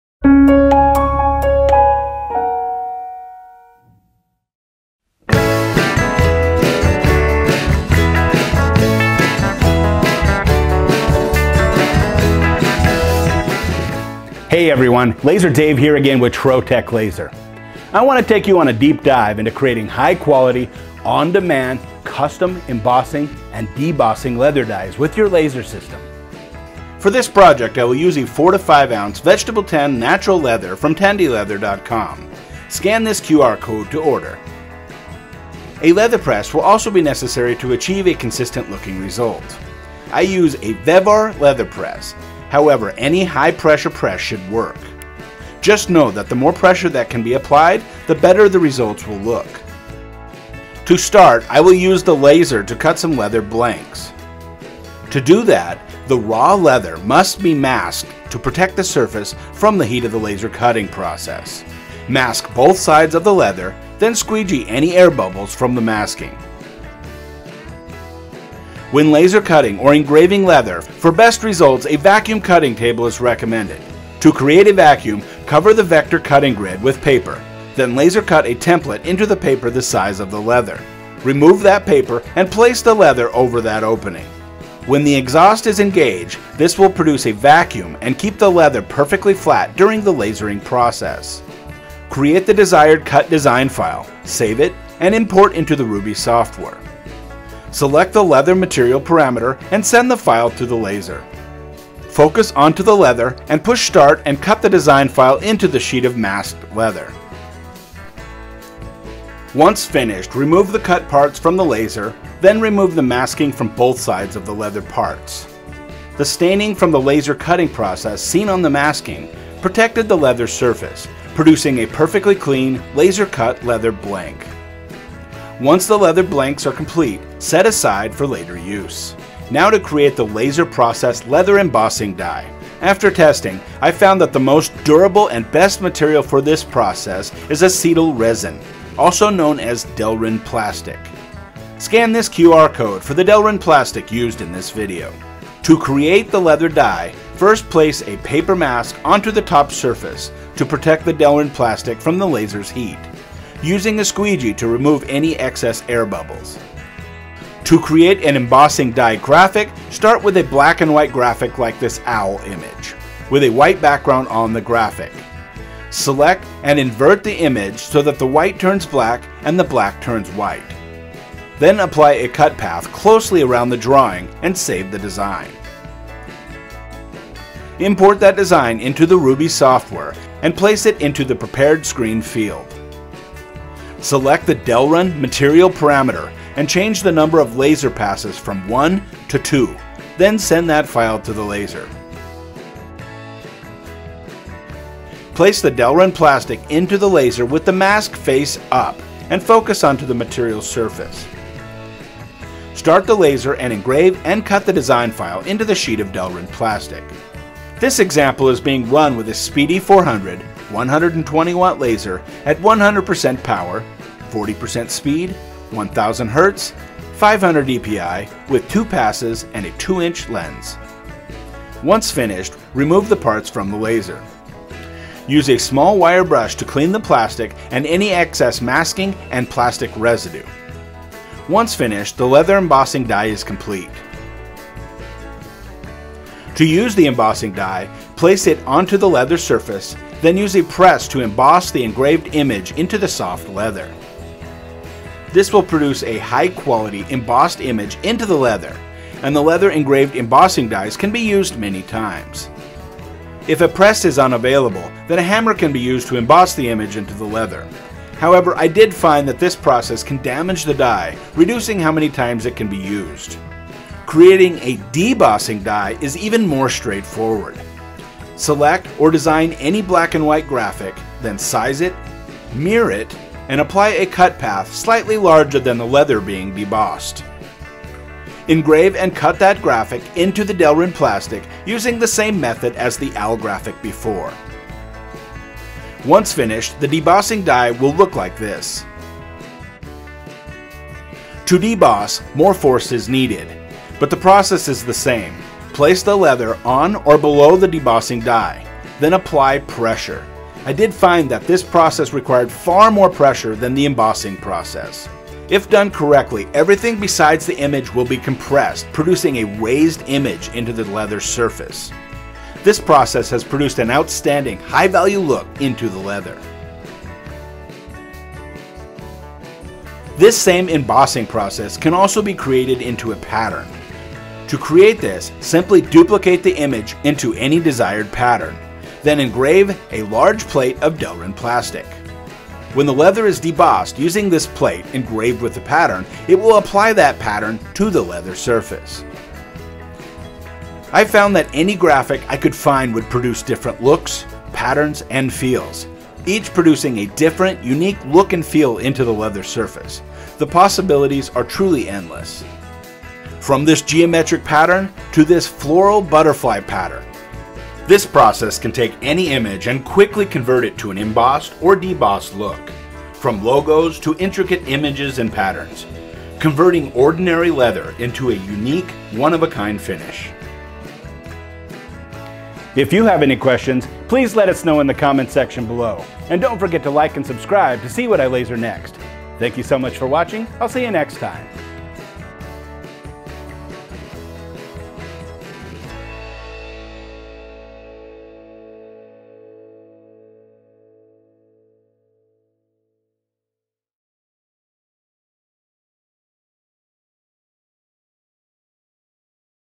Deep Dive with Laser Dave: Leather Embossing and Debossing Dies with Delrin Plastic
This month's deep dive offers an excellent solution for embossing and debossing leather by creating reusable dies with your laser system. Watch Laser Dave make unique dies using Delrin plastic.

Discover more ways to utilize your laser for leather cutting and engraving

Be the first to get updates!